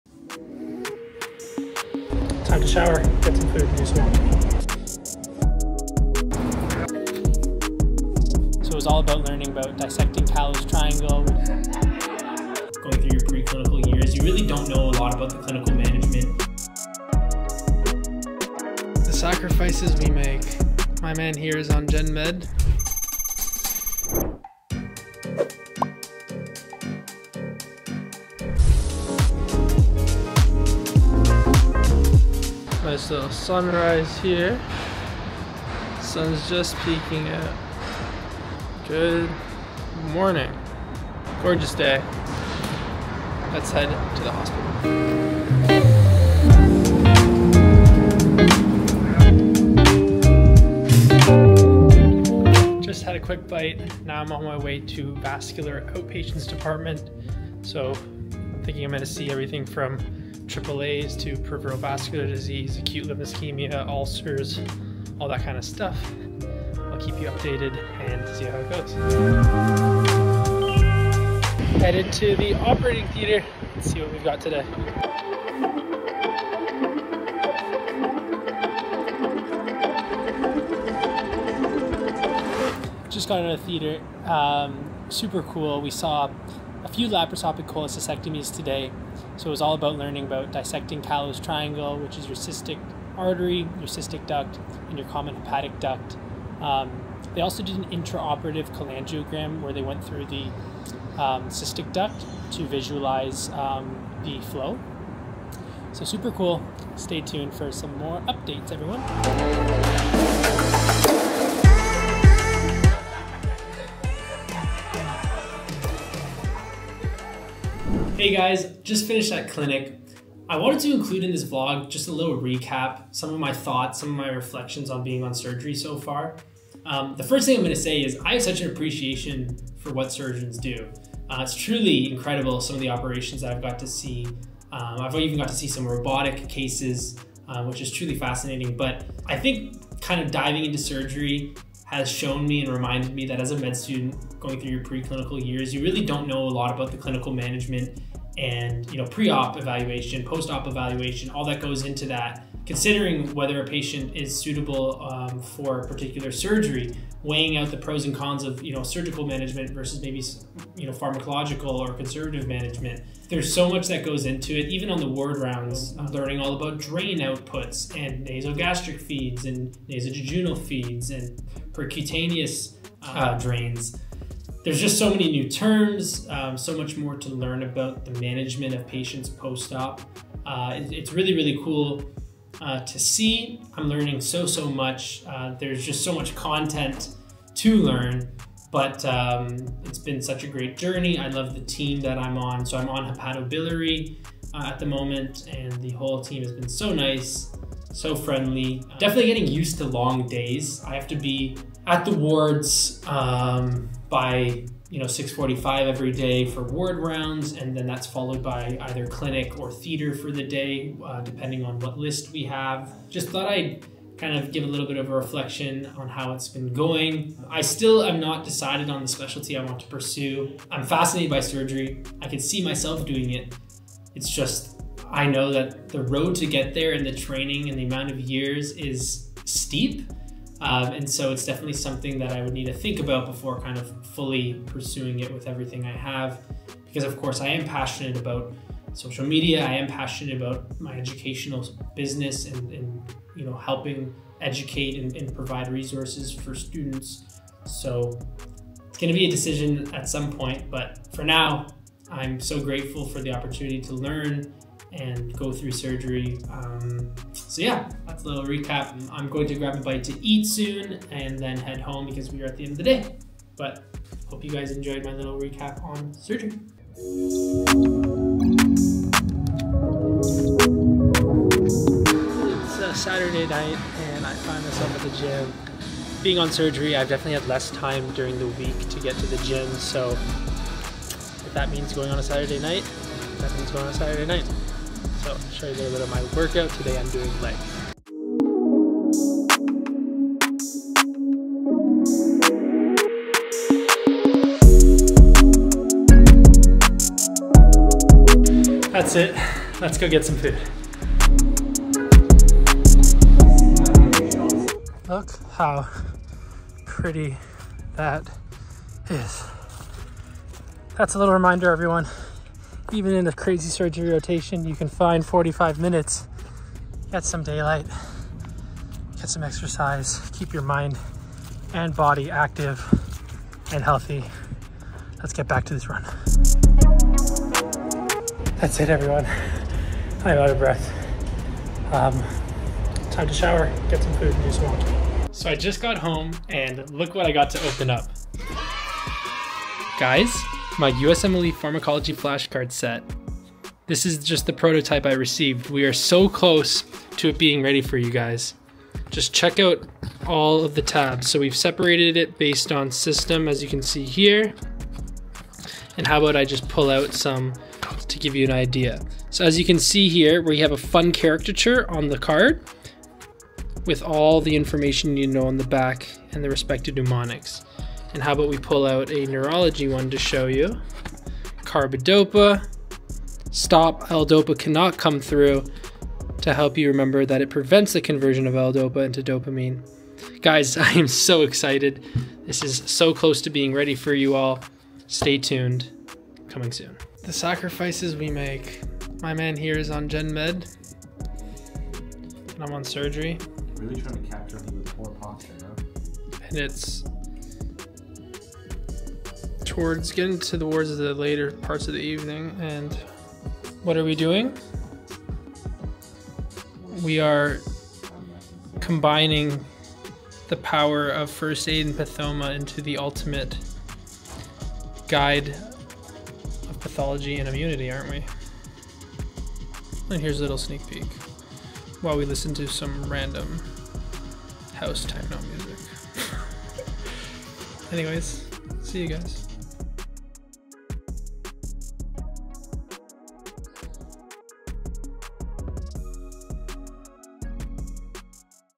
What Surgery Week Looks like in Medical School
In this video I take you through a week on Surgery Rotation looks like! I honestly learnt so much and capturing it all to share with you all made the week only that much more rewarding.